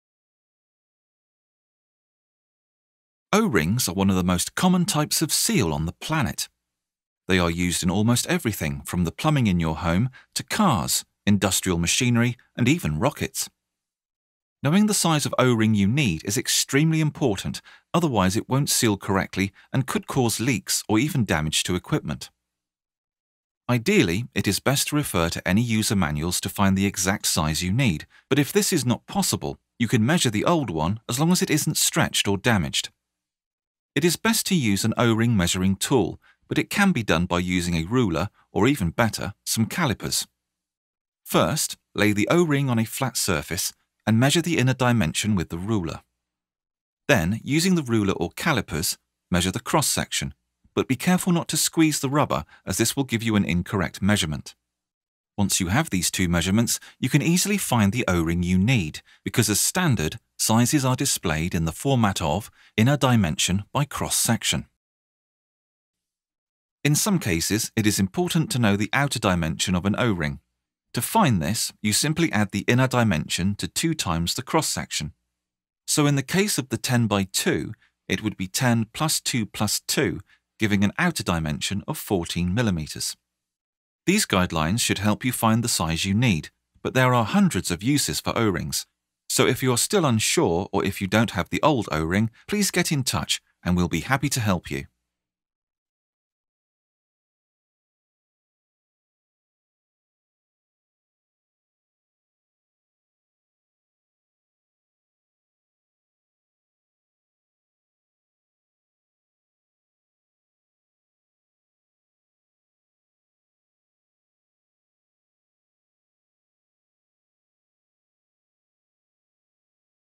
How to Measure an O Ring (with only basic tools)
In this video we teach you the basics about how to measure an o-ring with only basic equipment and what the dimensions mean.